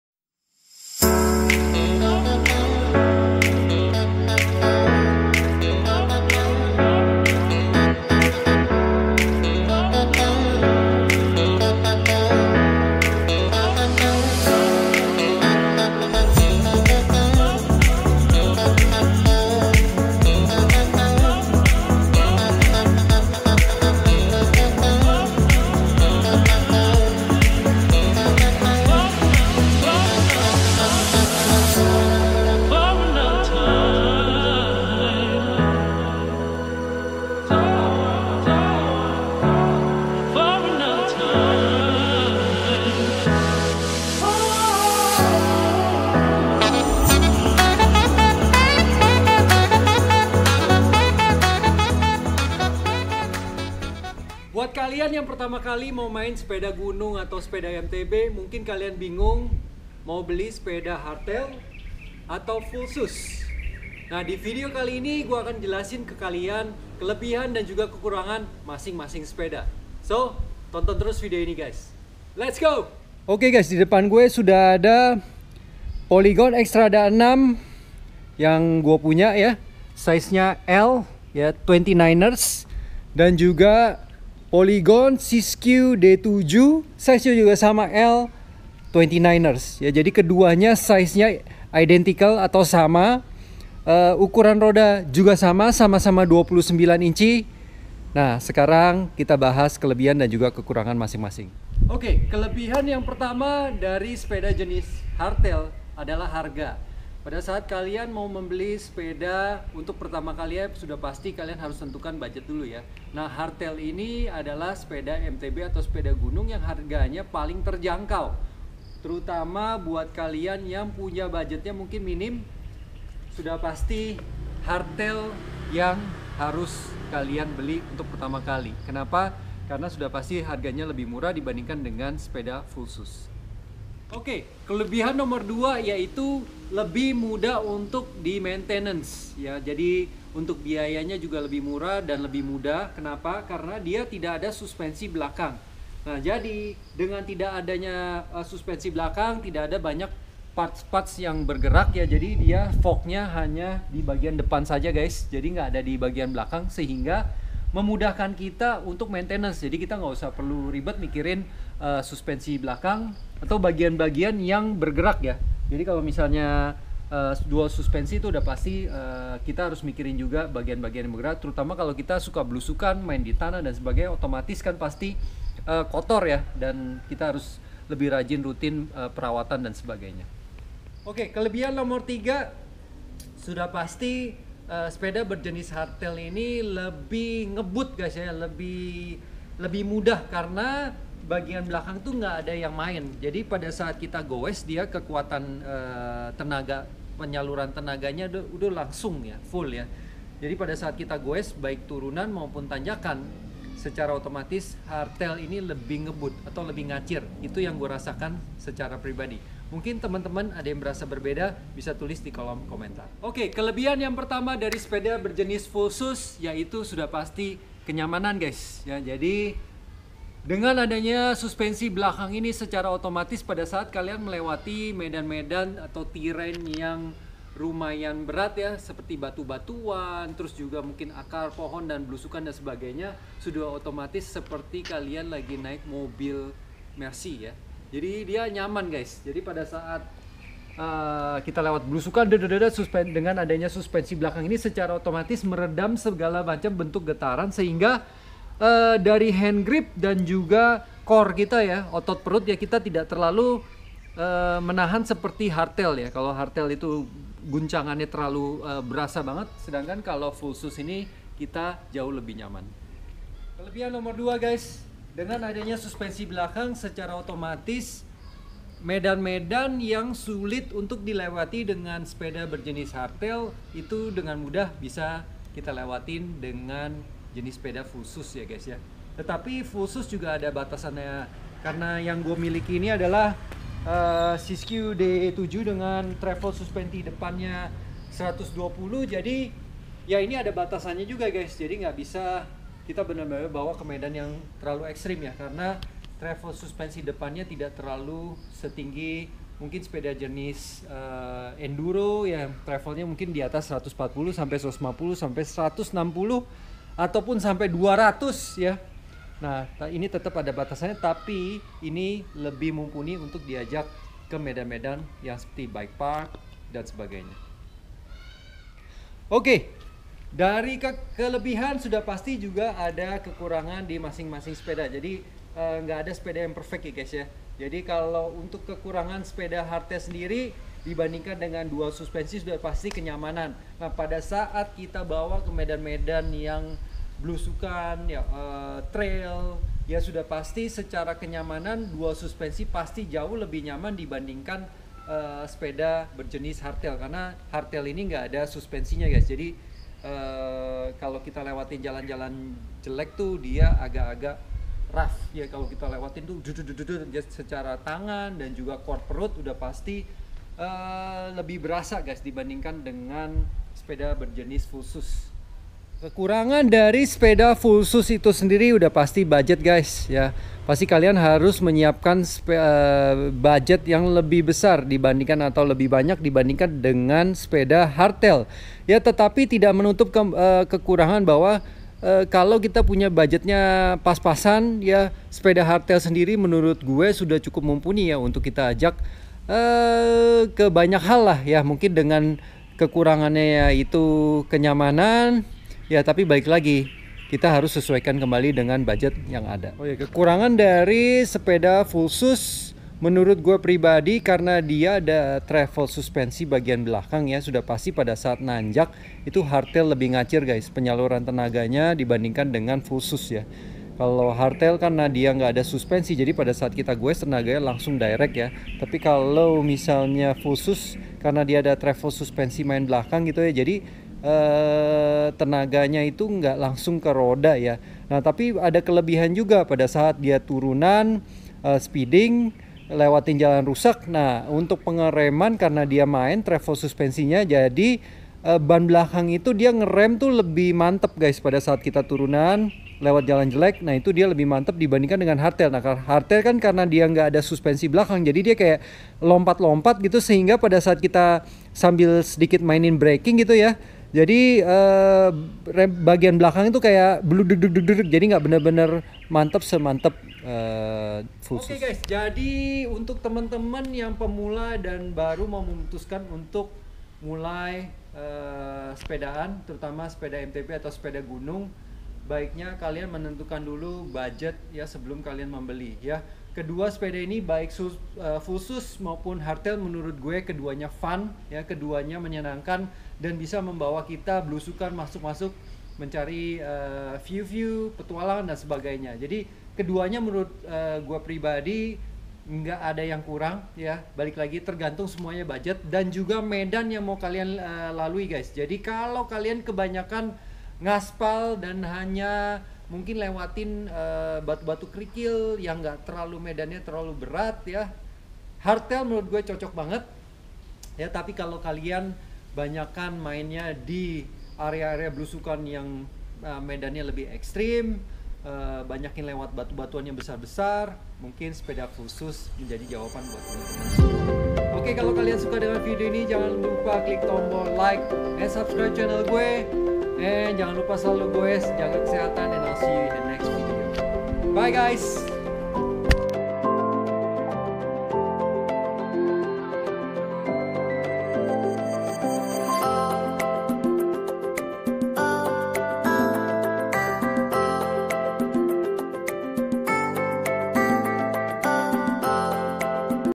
SEPEDA GUNUNG MTB HARDTAIL VS FULL SUSPENSION
Divideo gowes #76 kali ini gue bahas soal sepeda hardtail vs full sus guys. Hardtail sendiri menurut gue pribadi sepeda mtb yg paling terjangkau diluar dari hardtail yg menggunakan frame karbon pastinya lebih mahal atau merk luar, untuk full sus yg budgetnya minim atau dibawah 6jt kayanya kurang layak ya jadi mending skip dan ambil hardtail aja yg grupsetnya sudah cukup baik seperti menggunakan deore. Masing masing sepeda pastinya ada kelebihan dan juga kekurangan tergantung dari kebutuhan dan juga peruntukan kita, semoga dengan video ini bisa sedikit membantu kalian menentukan sepeda pertama yang akan kalian beli. Untuk masing masing sepeda sudah pernah gue review kalian bisa cek divideo sebelum ya guys. Kalau ada pertanyaan bisa kalian tanyakan dikolom komentar mohon maaf apabila ada salah salah kata 🙏🤘😁 semua review murni berdasarkan pengalaman pribadi yg gue sharing ke kalian.